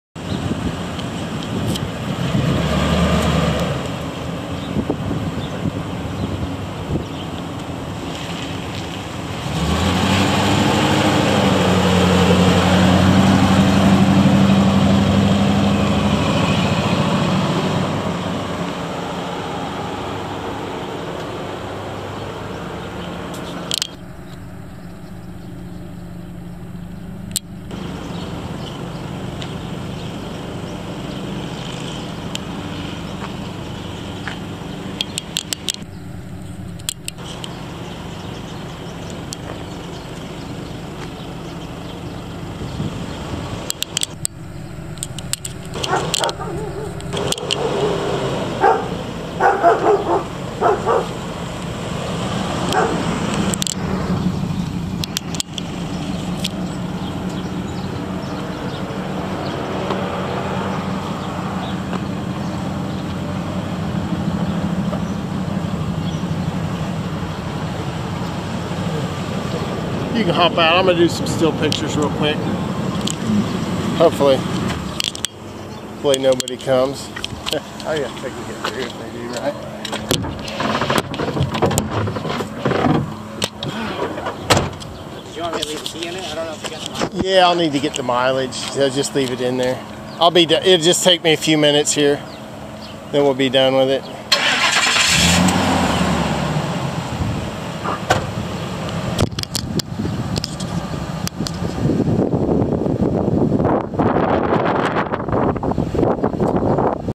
2004 Ford F250 Super Duty SuperCab pickup truck for sale at auction | bidding closes June 15, 2022
2004 Ford F250 Super Duty  SuperCab pickup truck
 Miles: 180,330 on odometer
 VIN: 1FTNX21L84EC25877
Engine
 Displacement: 5.8L V8 SOHC
 Cylinders: 8
 Fuel type: Gas
Transmission
 Automatic
 Four wheel drive
Interior
 AC, Heat
 Cruise control
Tires
 Size: 245/75R16
Notes
 Windshield chipped or cracked
Kansas  title